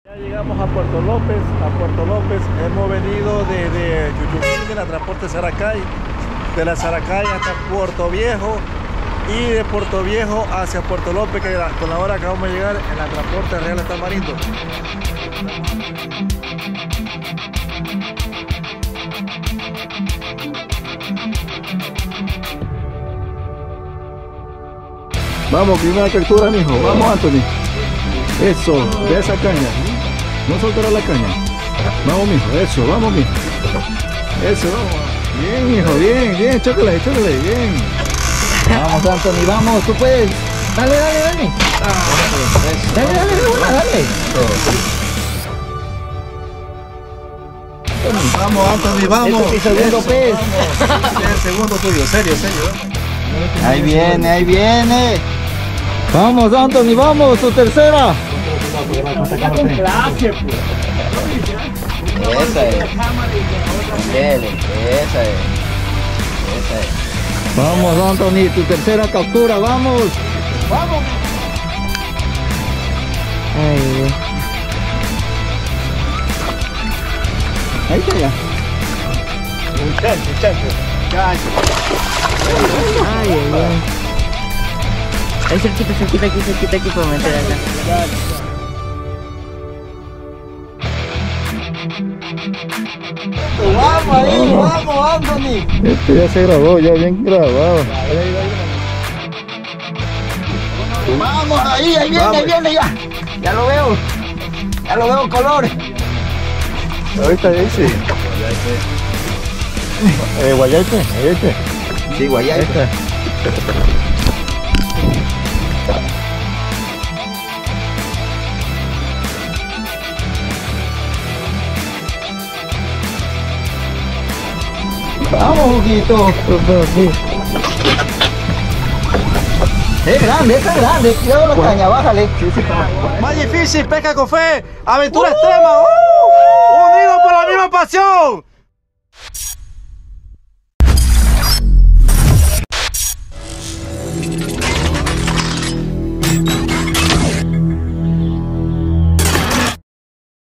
🎣 Adelanto del Próximo Video de la Semana 🔥
⚡DESCRIPCION ⚡
Saludos y bendiciones a todos los Colegas de Anzuelos, el video de hoy es el adelanto del próximo video que se estará publicando MAÑANA LUNES 17 DE ABRIL A LAS 18:00 p.m. (hora ecuatoriana), una pesca realizada en alta mar semanas atrás 🙌.

Credits:
Invitados: Guido Meneses, Denis Apolo y Capt. Blanco
Embarcacion: Magic Fishing Puerto Lopez
Grabación: Hugo Anchundia ( @pescaconfe1 )
Edición: Editor de Pesca Con Fe ( @pescaconfe1 )

~ creado por Pesca Con Fe
Lα cσpiα dε εstε vidεσ,
sεяα dεnunciαdα.